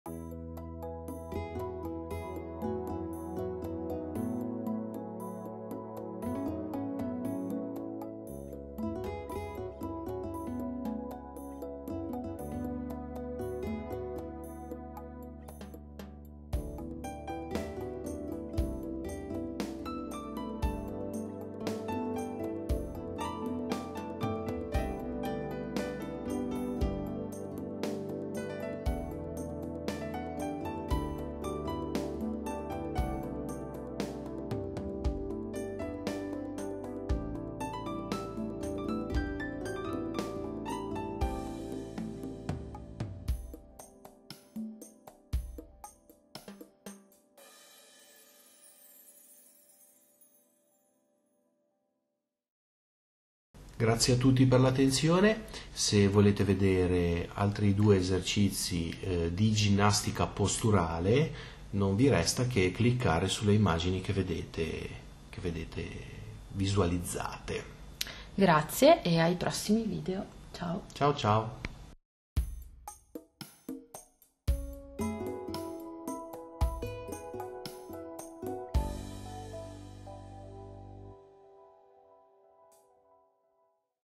Esercizi di mobilità per la schiena | Saluteinmovimento
Da - Un esercizio davvero efficace per mantenere la mobilità del rachide e della schiena, soprattutto a livello lombare e dorsale. Seguici anche su Facebook La ginnastica posturale per una schiena sana è su YouTube col canale Saluteinmovimento, sito web